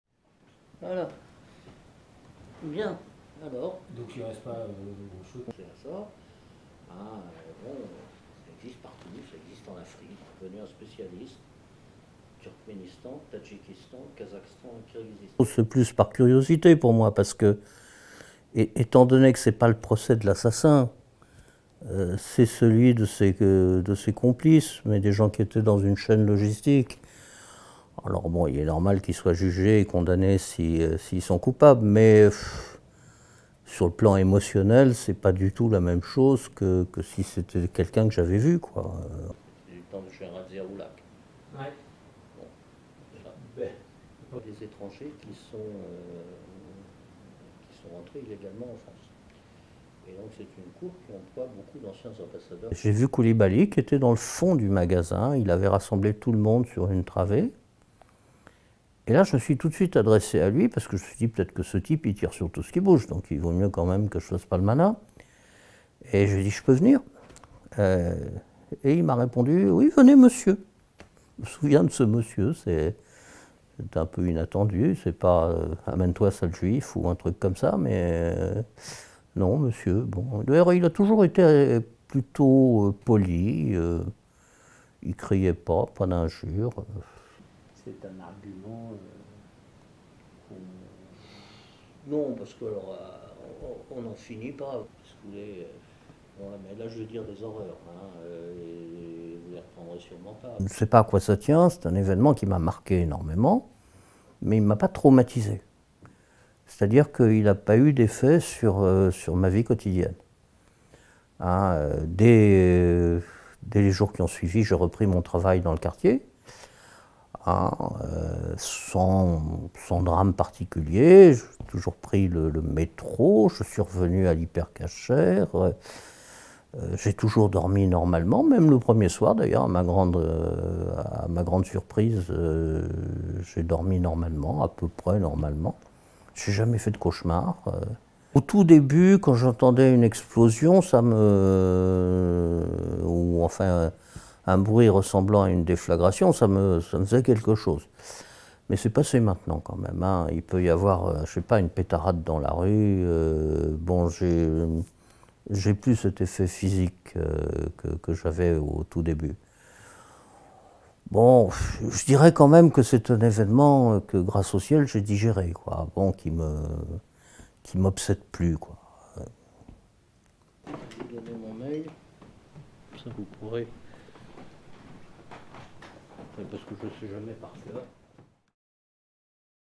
Hyper Cacher: 5 ans après, résilience et souvenirs glaçants d'un rescapé de l'attentat | AFP
"C'est un événement qui m'a marqué énormément mais il ne m'a pas traumatisé, il ne m'obsède plus": Alain Couanon, rescapé de l'Hyper Cacher, assistera au procès des attentats de janvier 2015 qui s'ouvre le 2 septembre. "Par curiosité" surtout, car cet ancien diplomate retraité de 73 ans confie s'être plutôt bien remis de la prise d'otage, même s'il en garde en mémoire des images terribles.